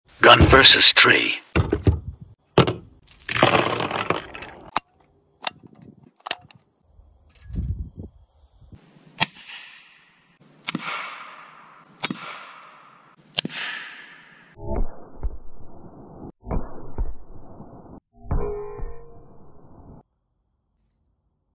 WK180C Gen2 VS Small Tree
Demonstration of Impact with different firearms.
Testing materials resistance within a secure environment at a private shooting range.
Recorded in a safe environment